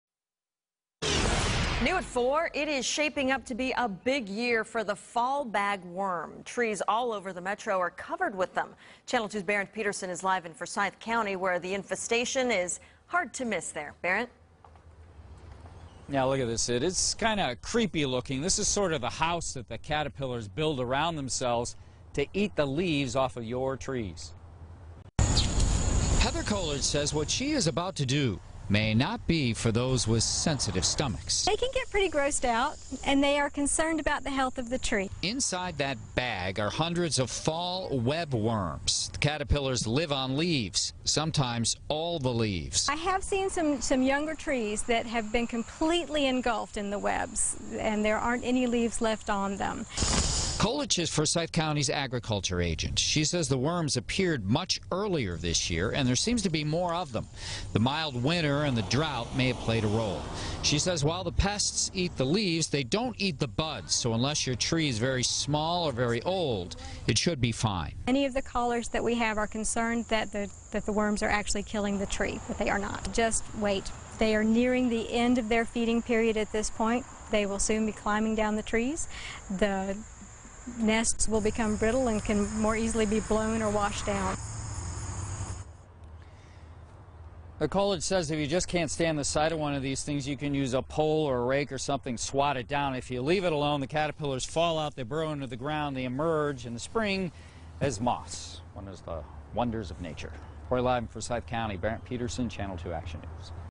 Creepy-looking web sacks are popping up all over Georgia
While the fall season won’t be here for another two months, these web worms are appearing much sooner because of the mild winters, allowing them to eat all the leaves off of your trees.